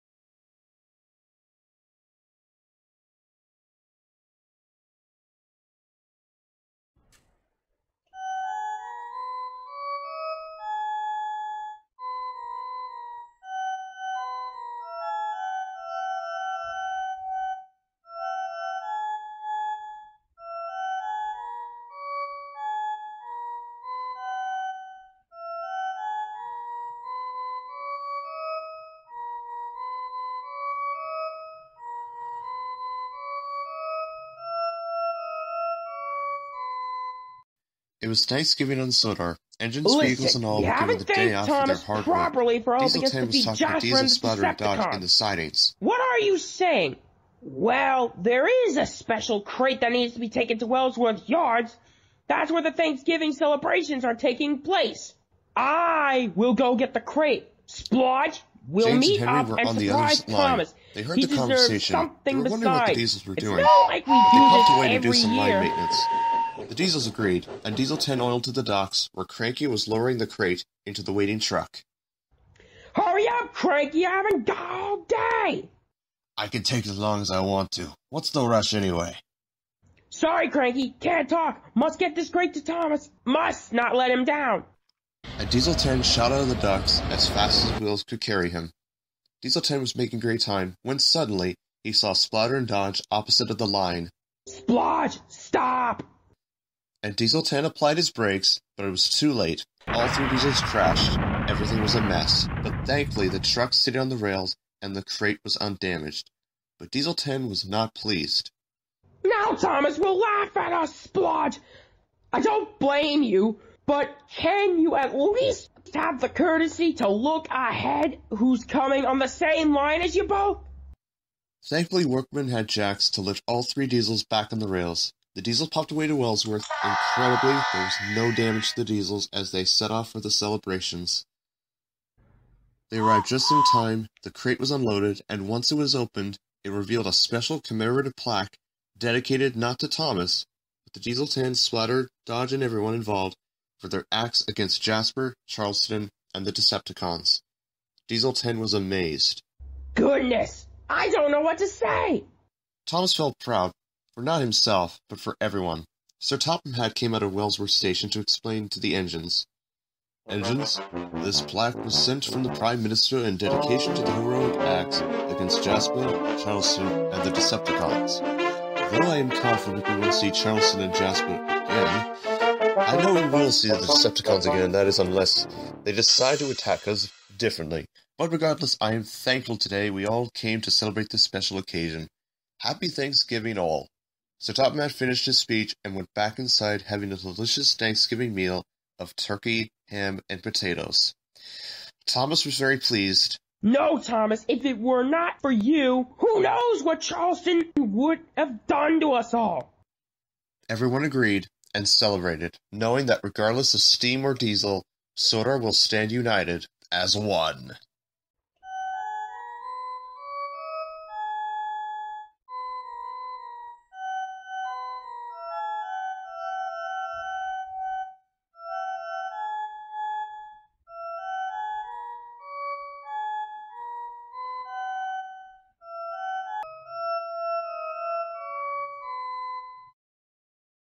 Thank You Thomas
Diesel 10 wants to repay Thomas for their work together months ago but will it go as planned

Roger Rubenstein As Diesel 10 Splatter And Dodge

Everyone Else As Myself

Sound Effects: Puffing Sounds: MallardFan62
Crash Sound Effect: Cinesound and Devran Hasan: Original Sound Effect: The Original Thomas And Friends Series

Other Sound Effects And Music Recreated By Me

An OptimusPrime2471 Productions